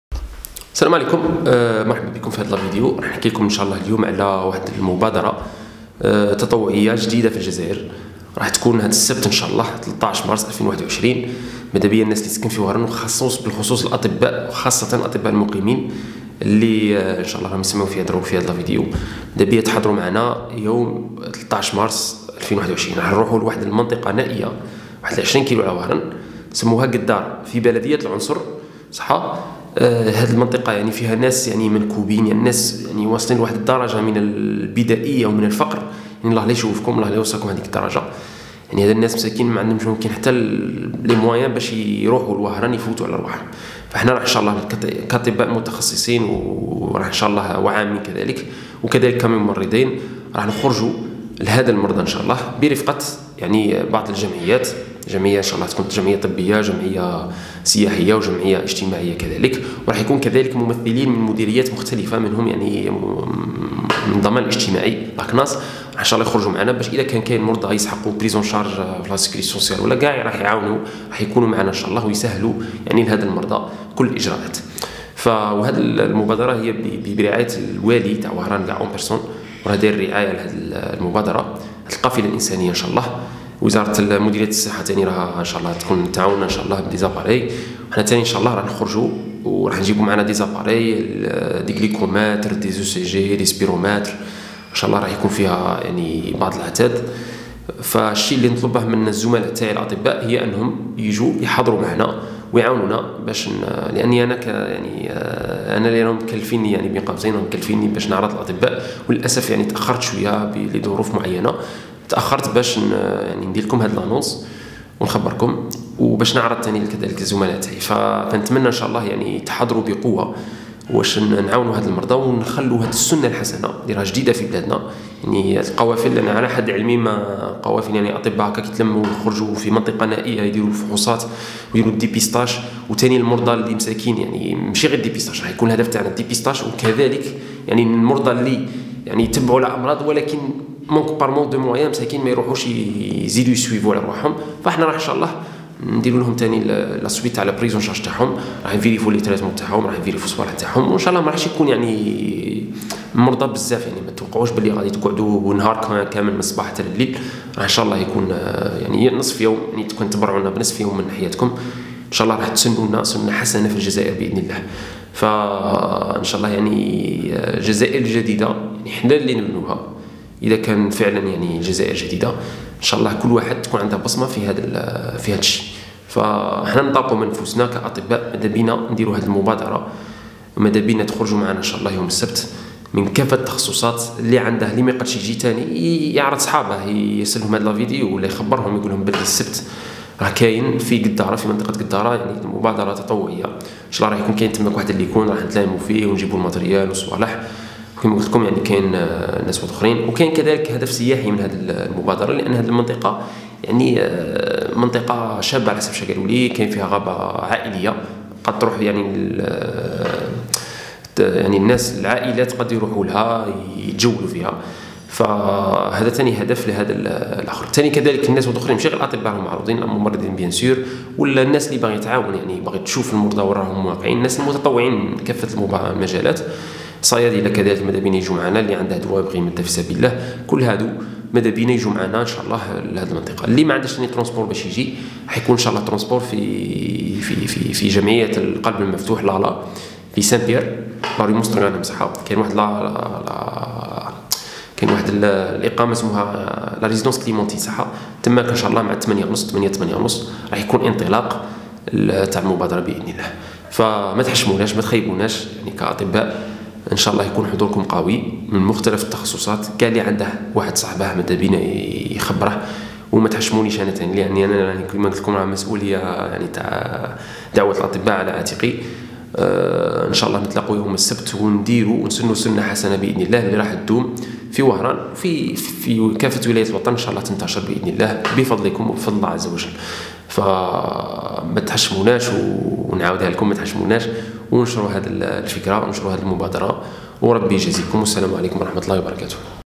La cardiologie comme spécialité pour les nouveaux lauréats
La cardiologie comme spécialité pour les nouveaux lauréats du concours de résidanat